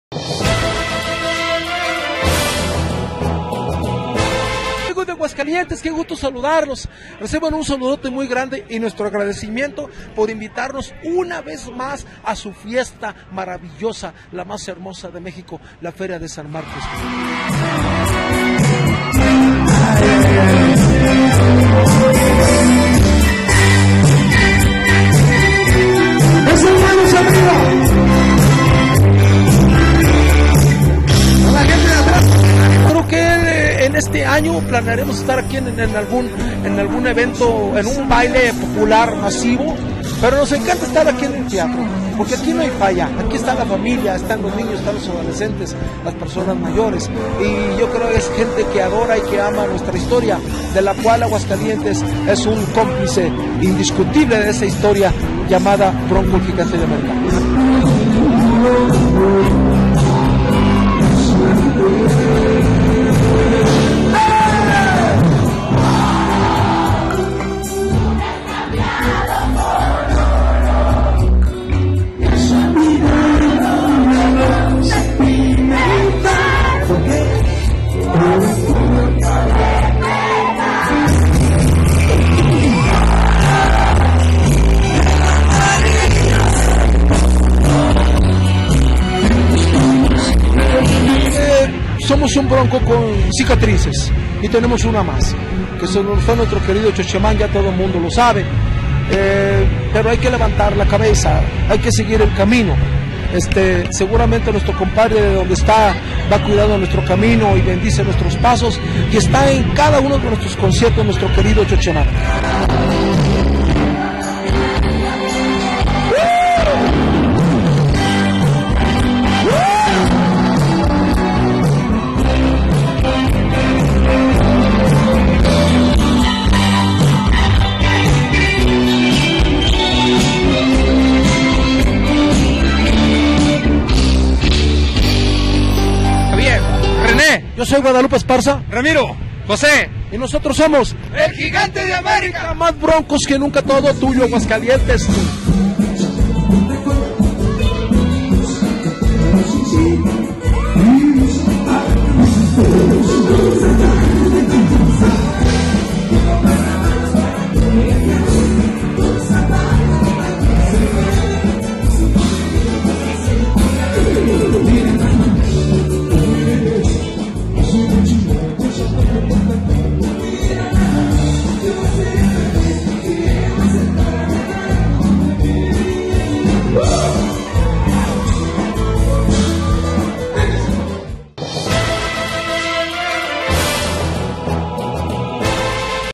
Latidos San Marcos: El Gigante de América
&nbsp;Vamos a ver el &eacute;xito de la presentaci&oacute;n de El Gigante de Am&eacute;rica en el teatro del pueblo de la FNSM 2013.